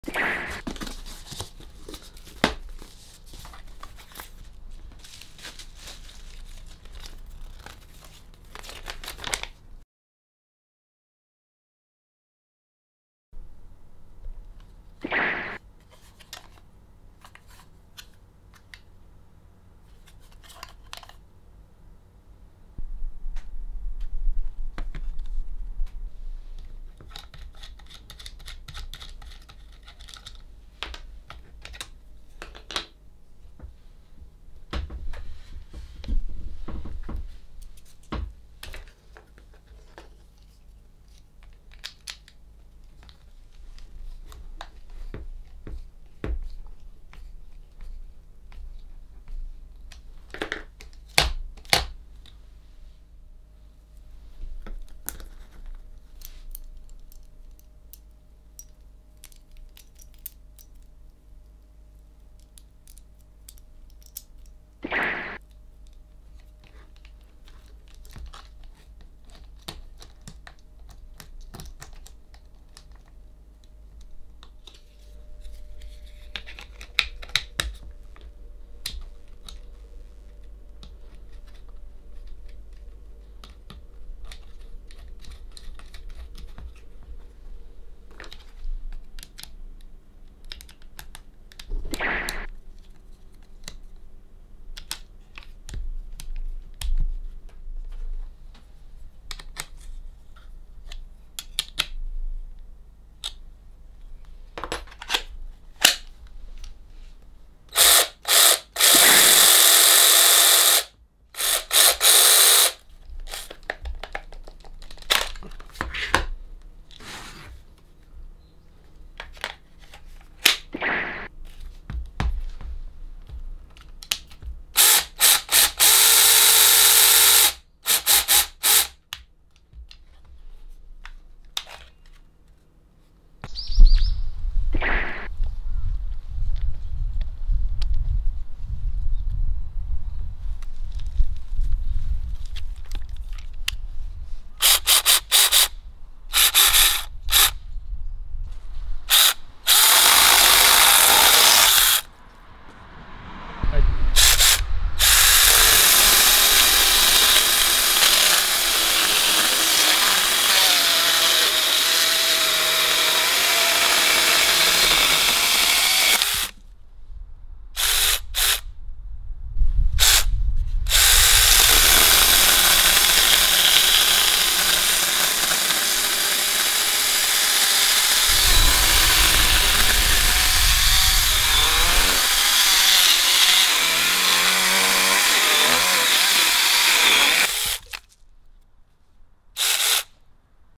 いつものマキタ互換ミニチェーンソー （Banggood提供品）
もう何度目でしょうか、マキタ互換のミニチェーンソーです、このタイプのチェーンソーはちょっと負荷をかけ過ぎただけでモーターから煙でます、ほっそい枝打ちにどうぞ

⭐️バングッドリンク

コード：BGa328e8（1/31迄）
割引率：40％オフ

コード：BG9ffac1（1/31迄）
適用価格：$10.99

コード：BGdc214a（1/31迄）
適用価格：$35オフ